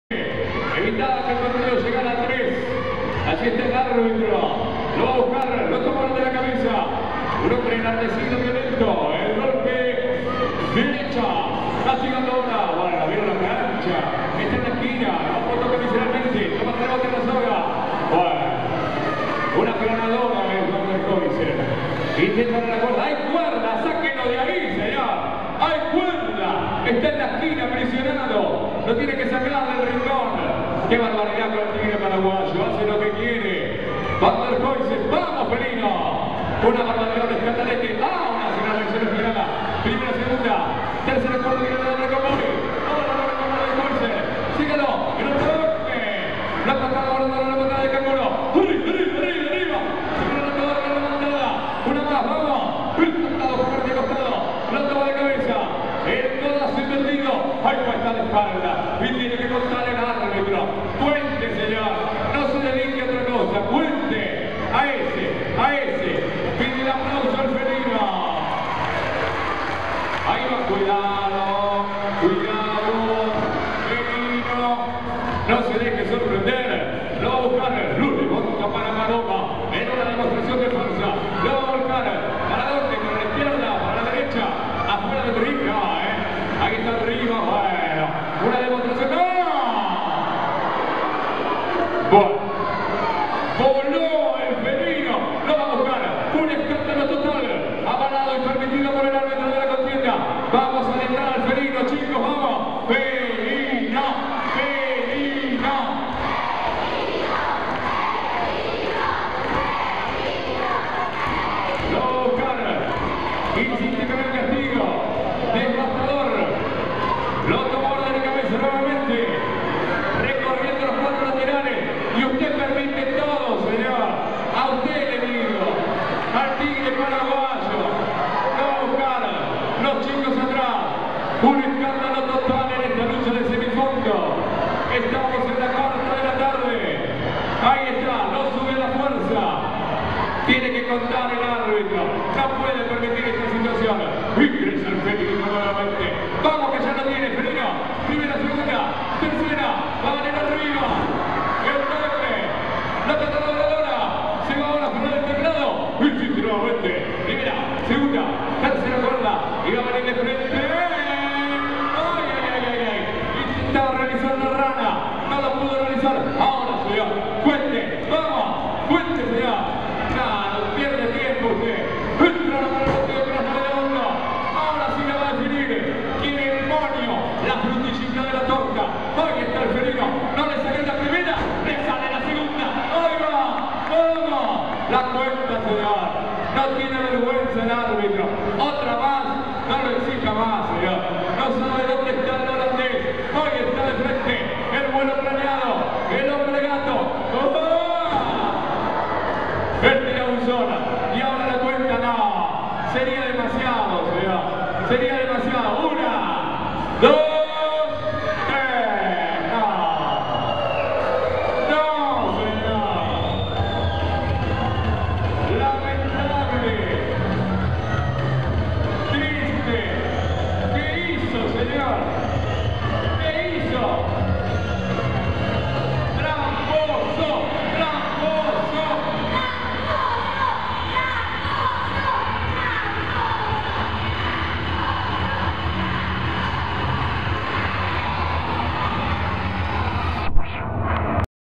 Van Der Hoisen vs Felino (fragmento)
ultima parte de la lucha del holandes Van der Hoisen contra el Felino.. para verlo!